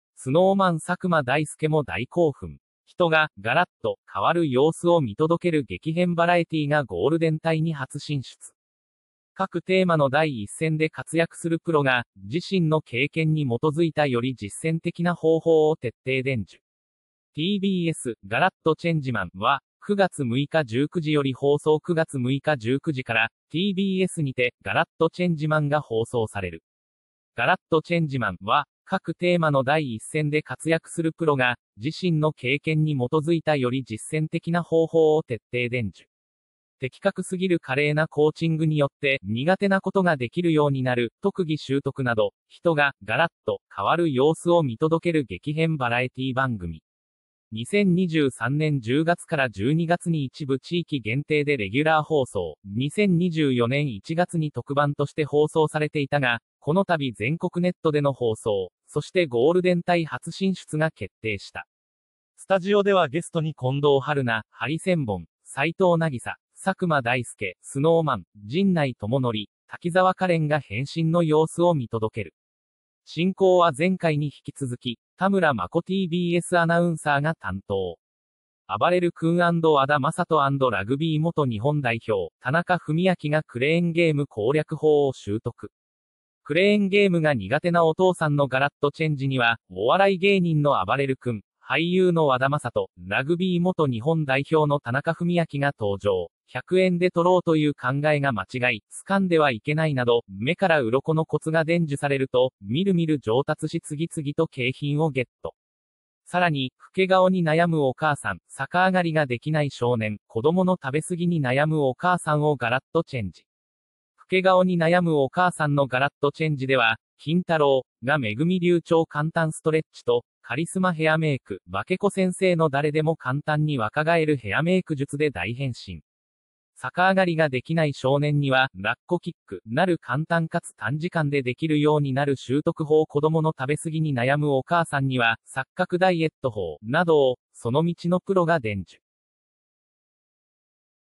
Snow Man佐久間大介も大興奮！人が“ガラッと”変わる様子を見届ける激変バラエティがゴールデン帯に初進出
■各テーマの第一線で活躍するプロが、自身の経験に基づいたより実践的な方法を徹底伝授！ TBS『ガラッとチェンジマン』は、9月6日19時より放送   9月6日19時から、TBSにて『ガラッとチェンジマン』が放送される。 『ガラッとチェンジマン』は、各テーマの第一線で活躍するプロが、自身の経験に基づいたより実践的な方法を徹底伝授。的確すぎる華麗なコーチングによって「苦手なことができるようになる」「特技習得」など、人が“ガラッと”変わる様子を見届ける激変バラエティ番組。 2023年10月から12月に一部地域限定でレギュラー放送、2024年1月に特番として放送されていたが、このたび全国ネットでの放送、そしてゴールデン帯初進出が決定した。 スタジオではゲストに近藤春菜（ハリセンボン）、齊藤なぎさ、佐久間大介（Snow Man）、陣内智則、滝沢カレンが変身の様子を見届ける。進行は前回に引き続き、田村真子TBSアナウンサーが担当。 ■あばれる君＆和田正人＆ラグビー元日本代表・田中史朗がクレーンゲーム攻略法を習得！ 「クレーンゲームが苦手なお父さん」のガラッとチェンジには、お笑い芸人のあばれる君、俳優の和田正人、ラグビー元日本代表の田中史朗が登場。「100円で取ろうという考えが間違い」「掴んではいけない」など、目からウロコのコツが伝授されると、みるみる上達し次々と景品をゲット！ ■さらに「老け顔に悩むお母さん」「逆上がりができない少年」「子どもの食べ過ぎに悩むお母さん」をガラッとチェンジ！ 「老け顔に悩むお母さん」のガラッとチェンジでは、キンタロー。がMEGUMI流超簡単ストレッチと、カリスマヘアメイク・化け子先生の誰でも簡単に若返るヘアメイク術で大変身。 「逆上がりができない少年」には“ラッコキック”なる簡単かつ短時間でできるようになる習得法、「子どもの食べ過ぎに悩むお母さん」には“錯覚ダイエット法”などを、その道のプロが伝授！IMPORTANT!